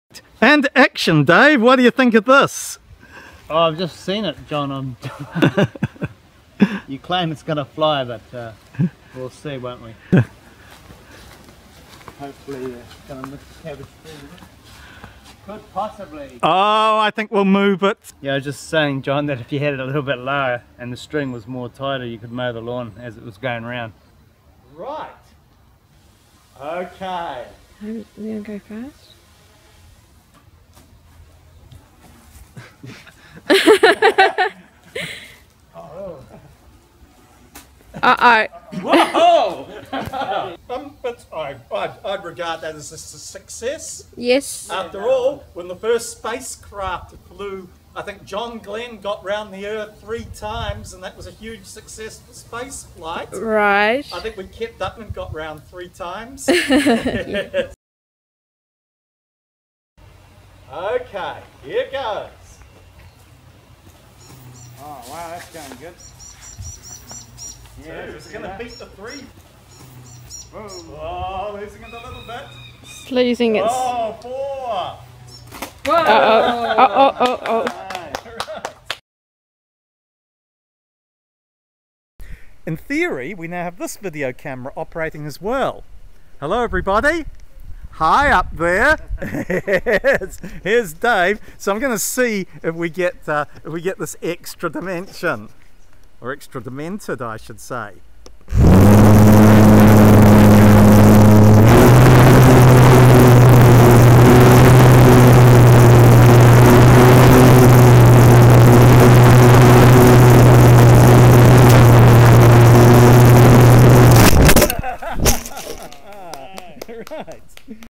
Electric Toothbrush Aircraft - Part 4 - Fly Toothbrush, Fly!
First flight gets 3 times around the pole. Flight02 does 4 circuits and we have a "PlaneCam" operating.

Part 4 in this series of using the power module from an electric toothbrush to fly a model aircraft. Part of efforts to come up with interesting STEM activities for mainly 10-15 year olds. More about that at:

Uncontrolled free flight is a bad idea with the remarkably powerful electric toothbrush motor and battery module which can deliver full power for about 4 min. Simple control here is by "Round the Pole" with a tether line.

This garden is at the bottom of a little valley and sheltered from the wind. IMO a small wind effect is having a big effect on flying height.